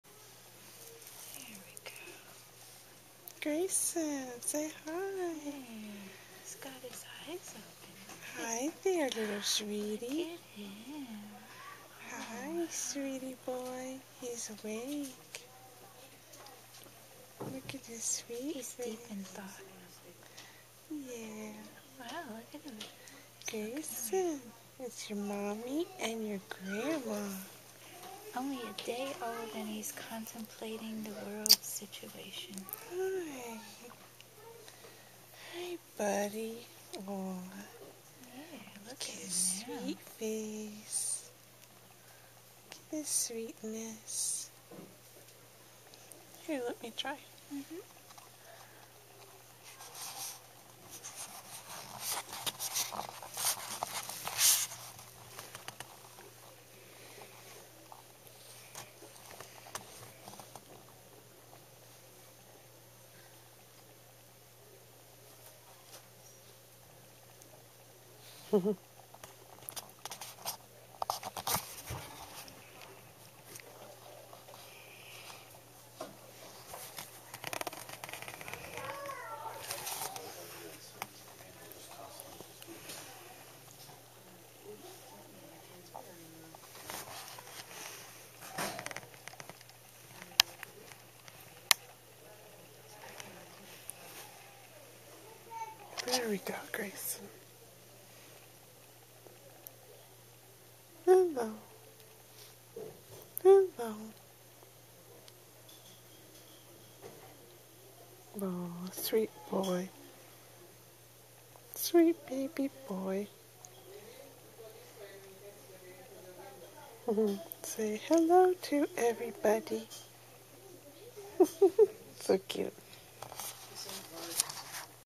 Grayson Danger newborn baby eyes open
grayson newborn opens his eyes to say hello